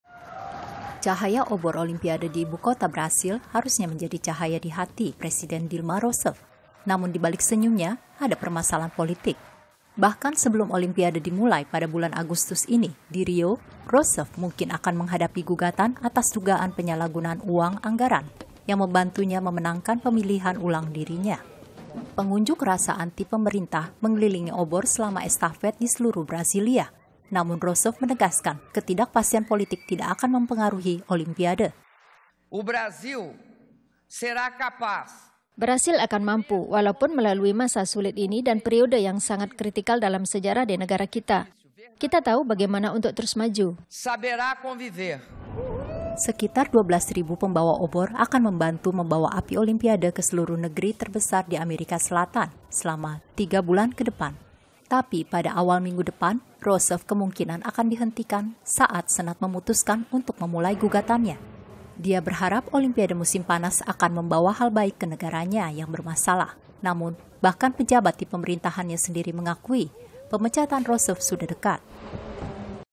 Obor Olimpiade Telah Dinyalakan di Brasil
Presiden Dilma Rousseff menyalakan obor Olimpiade di ibukota Brasil, dan berjanji bahwa kekacauan politik yang melanda bangsa-nya tidak akan merugikan Olimpiade pertama yang diadakan di Amerika Selatan. Harapannya Olimpiade Musim Panas akan memberikan hal baik ke negaranya.